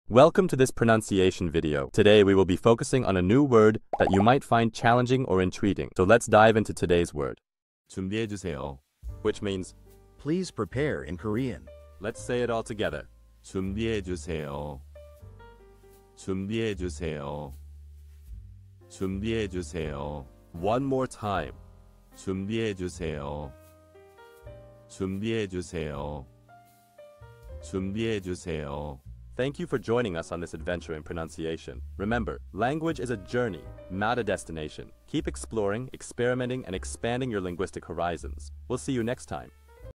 How to pronounce junbihaejuseyo | 준비해주세요 (Please prepare in Korean)
Master the Pronunciation of 'junbihaejuseyo | 준비해주세요' - which means : Please prepare in Korean 📝 with @PronunciationKorean 🎓🗣️ - [Your Guide to Korean]

Learn to pronounce 'junbihaejuseyo' perfectly in Korean with our easy and engaging tutorial by @PronunciationKorean! 📖📽️ In this comprehensive video, we provide you with native speaker examples 🗣️👂 and phonetic breakdowns 🎵 to ensure you get the pronunciation just right.